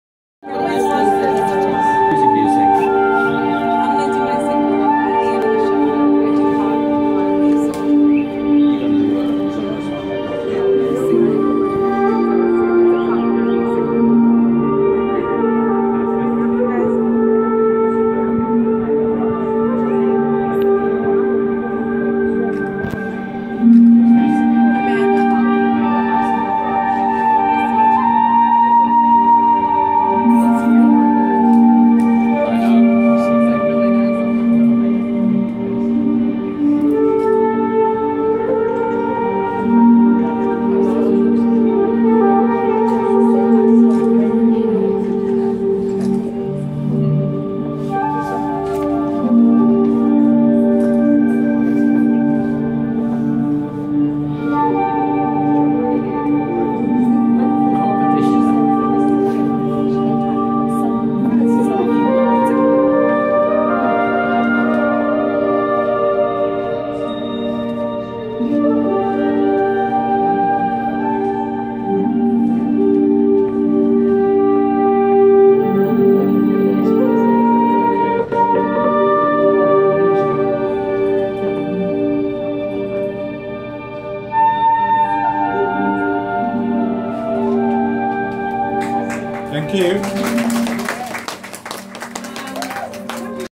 旗袍 Our Collective Fashion Show
表演: 旗袍美人, 面對人生, 悲歡離合内僉而優雅的華麗轉身。那内在的自我修養提升。也是後來解說那中國陰陽五行醫典"黄帝内經"宇宙觀的思想工作。

( 我那个模特兒表演是有話劇角式演出的, 比較誇張些的, 所以不是一般如其他。也要解釋中醫學分面, 所以看來較認真  又要英文演出難度较高。)